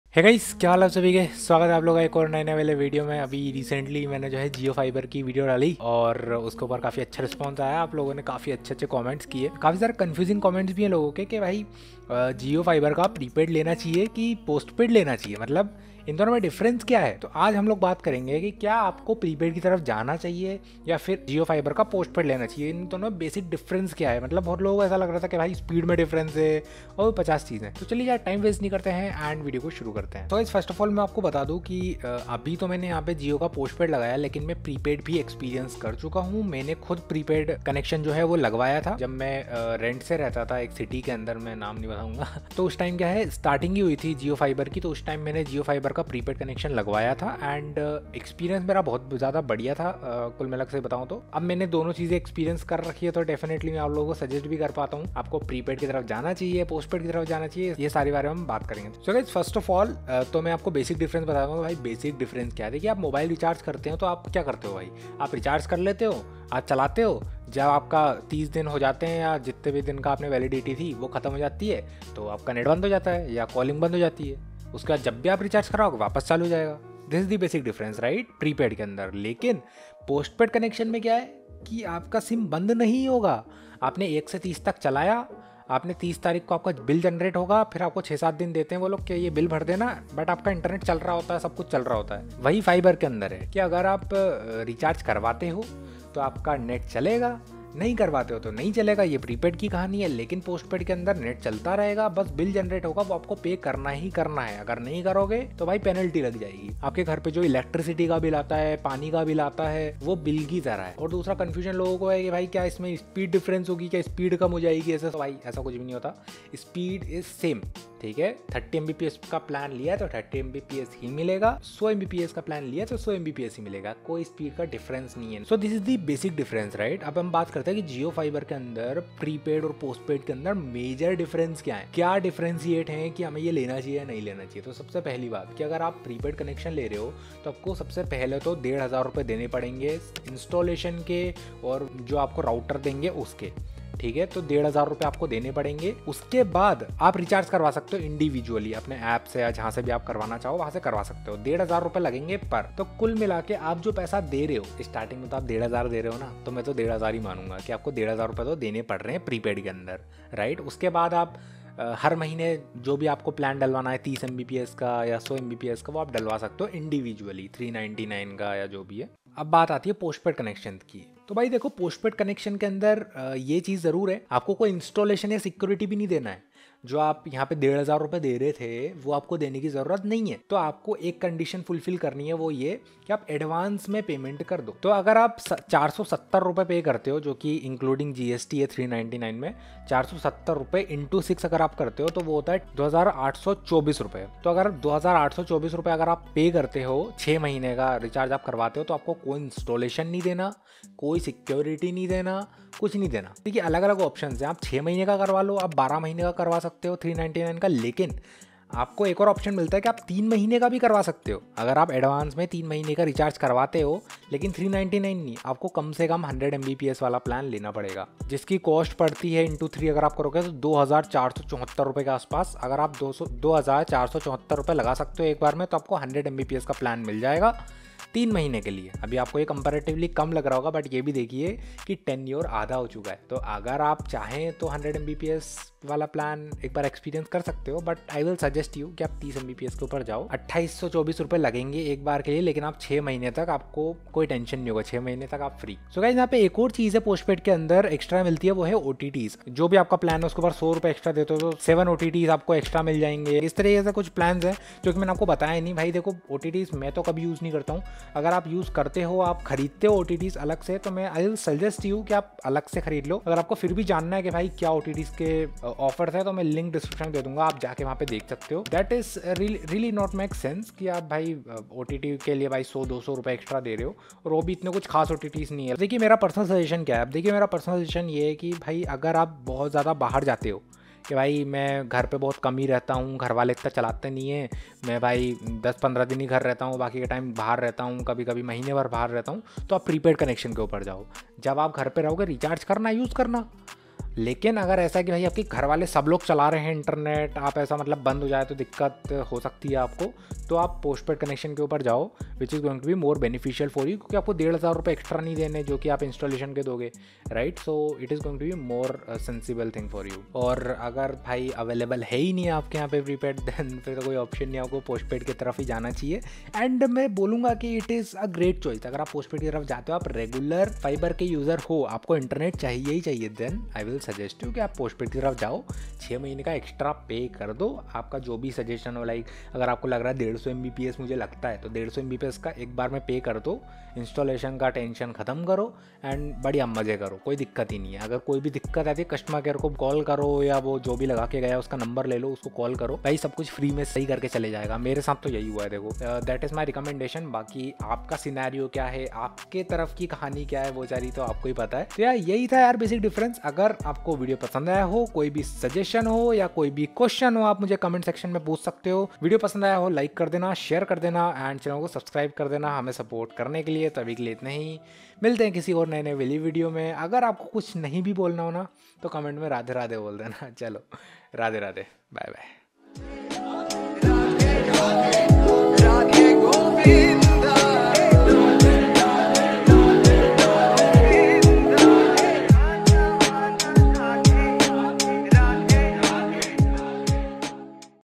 Jio Fiber Prepaid vs Postpaid | Jio Fiber Installation Charge 2024
In this video, we compare Jio Fiber prepaid and postpaid plans, including installation charges for 2024. We discuss the benefits and drawbacks of each option, helping you make an informed decision about which Jio Fiber plan is right for you. Whether you're considering Jio Fiber for your home or business, this comprehensive comparison will guide you through the key factors to consider.